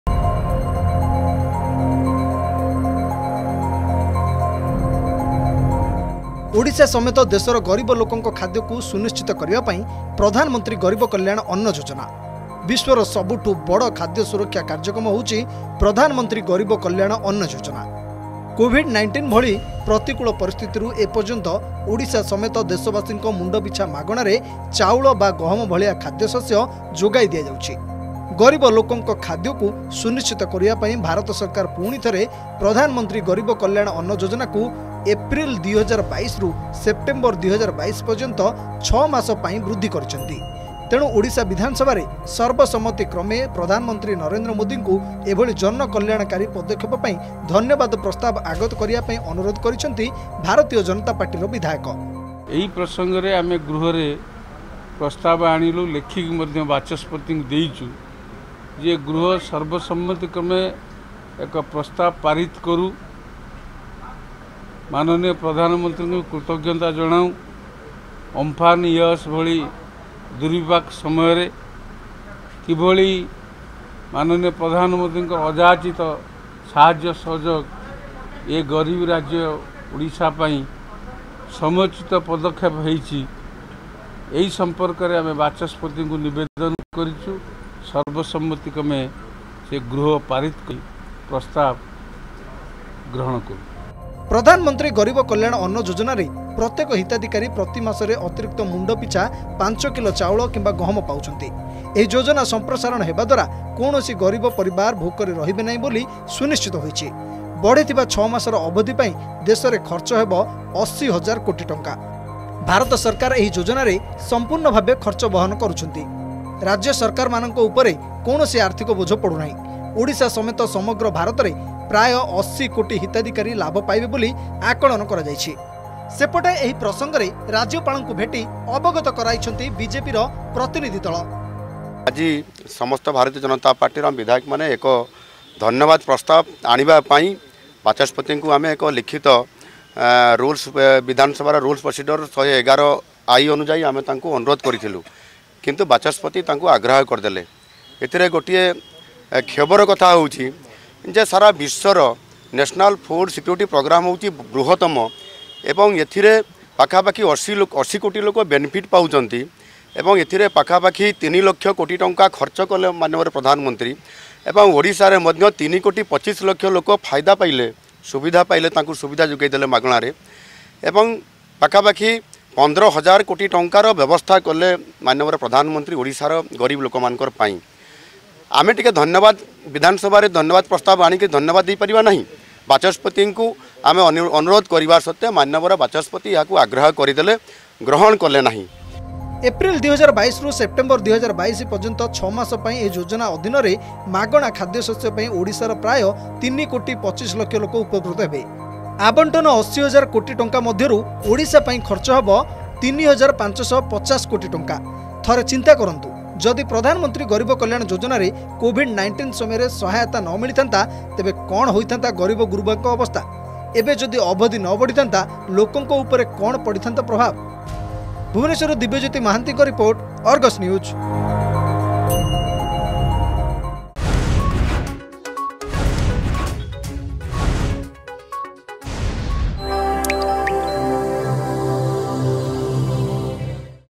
Pradhan Mantri Garib Kalyan Anna Yojana Is World's Largest Food Security Programs
Pradhan Mantri Garib Kalyan Anna Yojana Is World's Largest Food Security Programs. 3.25 crore beneficiaries are from Odisha. BJP Wants Odisha Assembly to Thank the PM for this Endeavor.